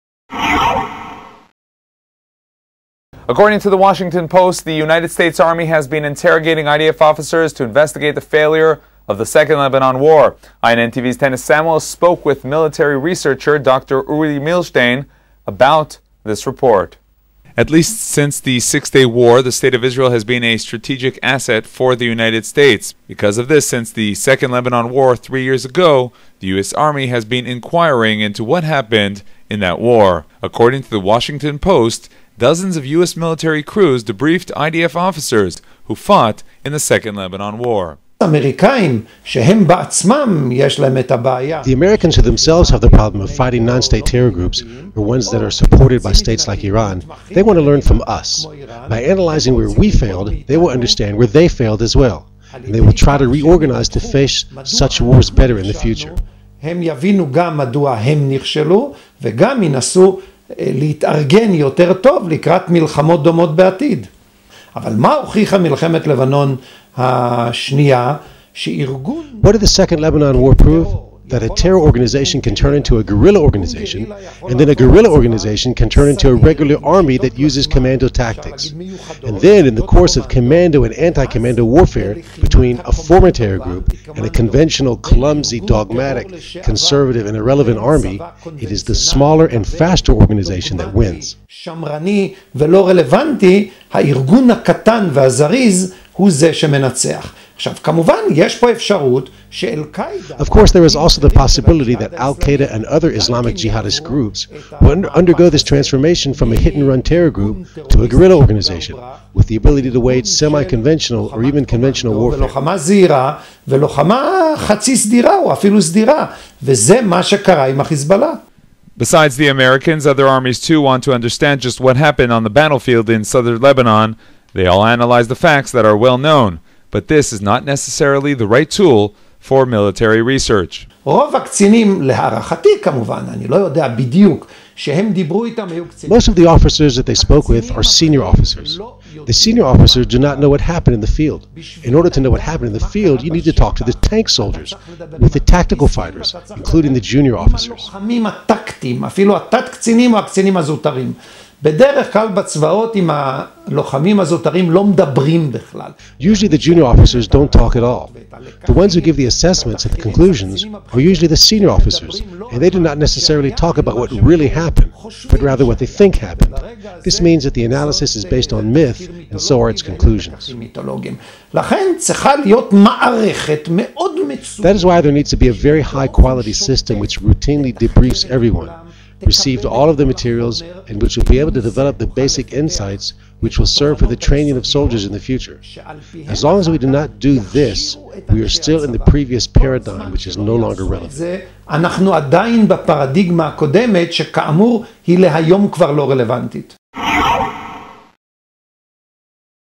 US Army Debriefed IDF Officers
Military researcher Dr. Uri Milstein relates to the report that the US Army investigated the results of the 2nd Lebanon War. As seen on Arutz Sheva.

IsraelNationalNews.com